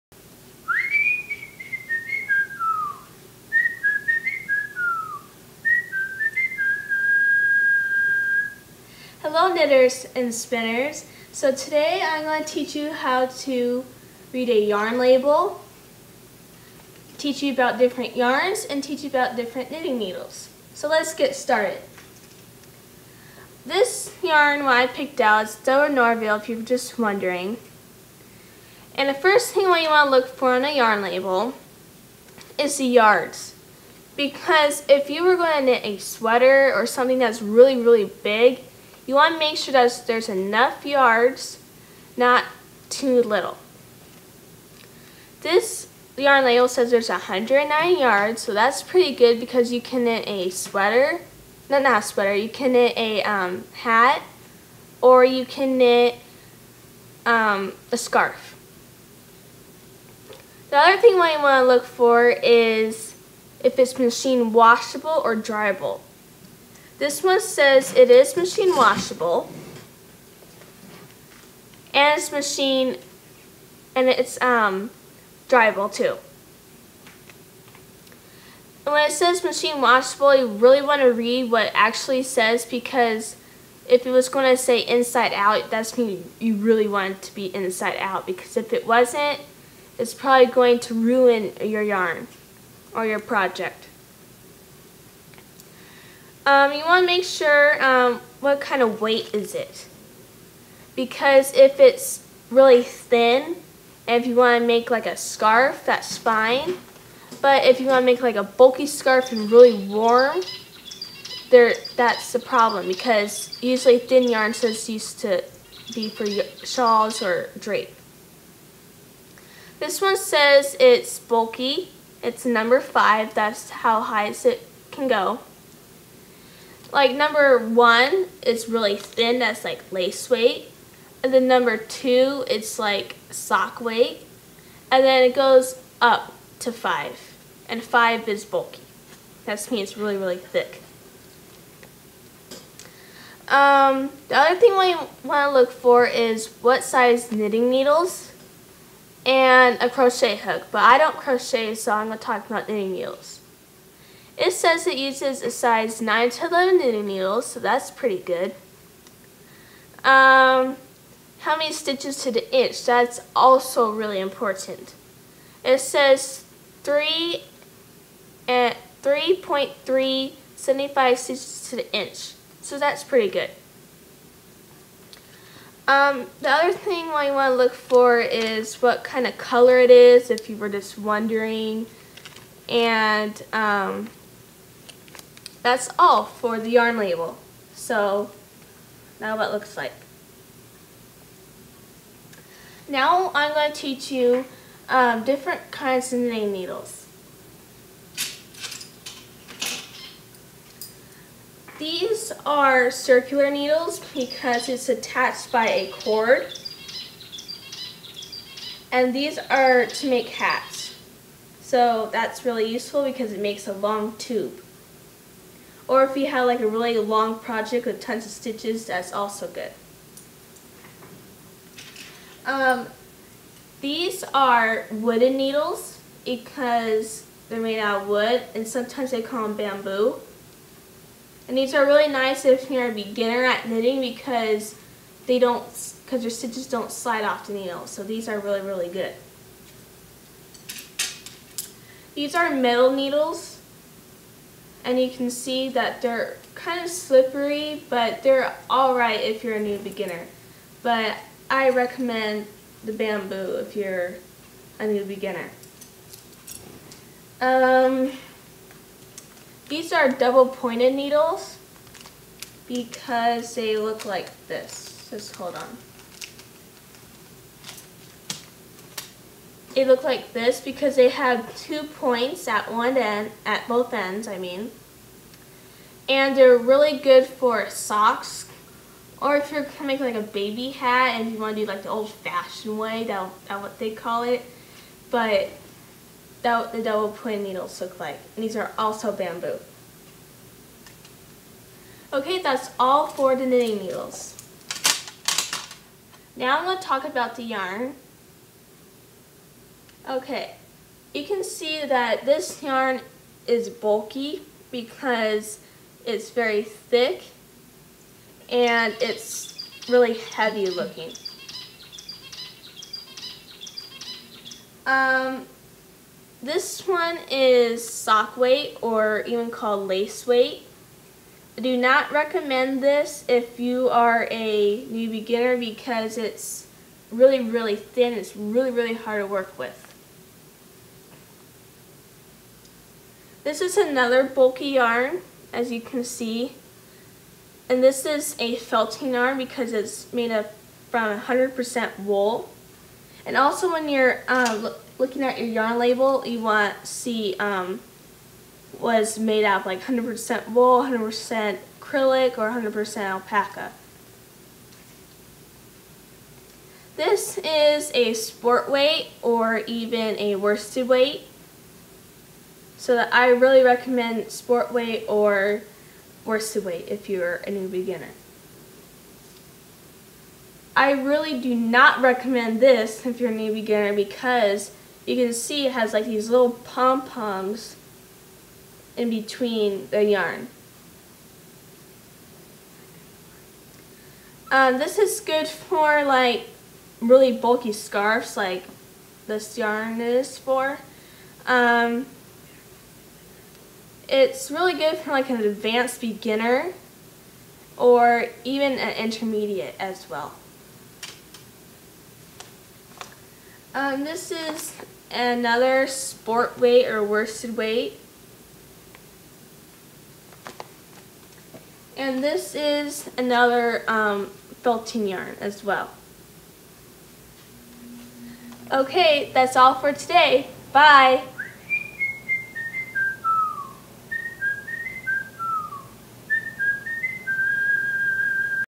1- Getting started
I am demonstrating how to read a yarn label , show you different yarns , and knitting needles.